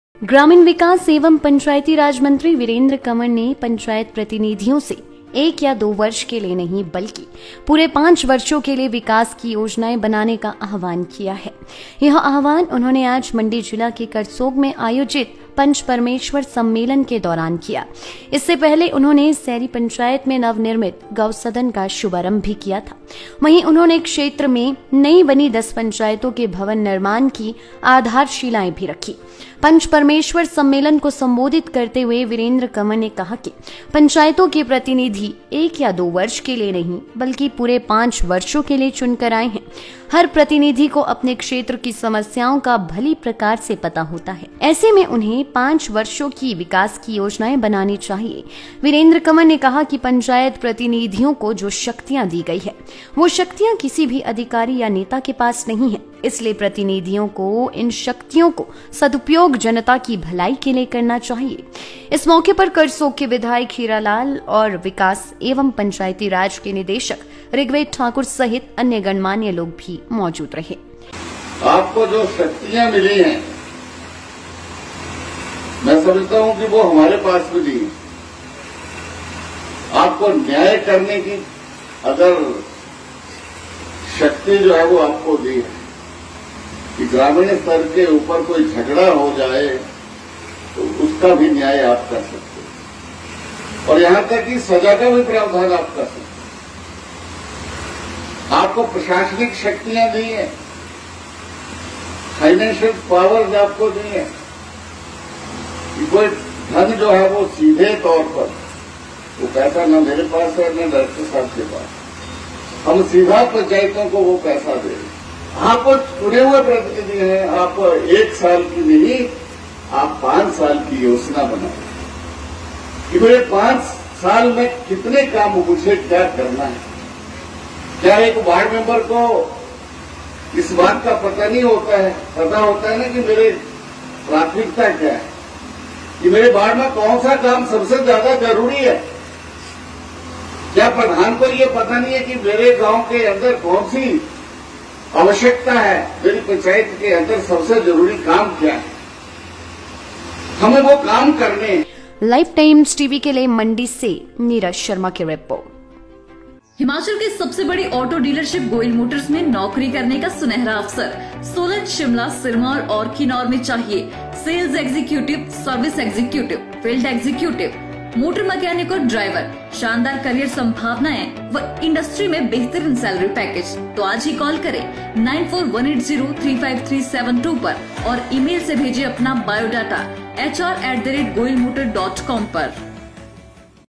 Mandi: Elected Panchayat representatives should make plans for the development of five years.
मंडी : पांच वर्षों के विकास की योजनाएं बनाएं चुने हुए पंचायत प्रतिनिधि।
करसोग में बोले ग्रामीण विकास एवं पंचायती राज मंत्री वीरेंद्र कंवर...
10 नवगठित पंचायतों के पंचायत घरों की रखी आधारशिला..
सेरी पंचायत में गौ सदन का भी विधिवत रूप से किया शुभारंभ
Rural Development and Panchayati Raj Minister Virendra Kanwar said in Karsog...
Foundation stone laid for Panchayat Ghar of 10 newly formed Panchayats..
Cow Sadan was also duly inaugurated in Seri Panchayat